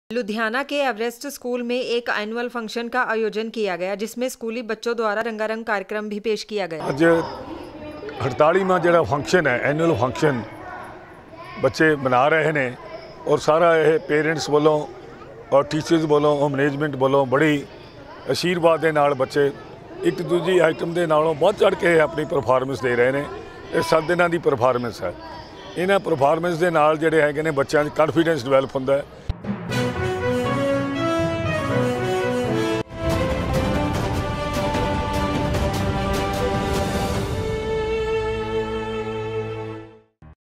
Times of Punjab एवरेस्ट स्कूल में एनुअल फंक्शन का आयोजन किया गया
एवरेस्ट स्कूल में  एनुअल फंक्शन का आयोजन किया गया